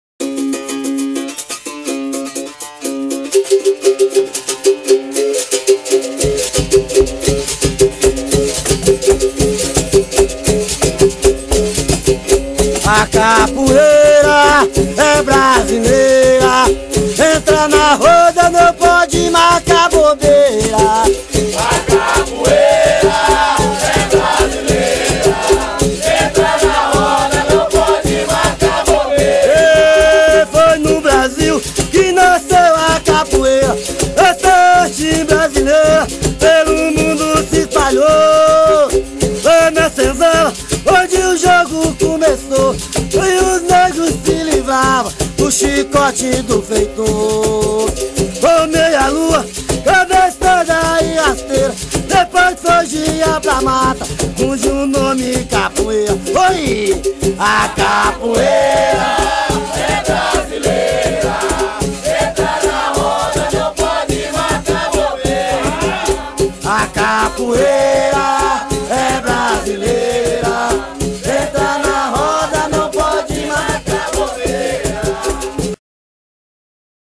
Mestrando Padeiro
Mestrando Padeiro jogando primeiro com Mestre Adilson e depois com o Mestre Maurão...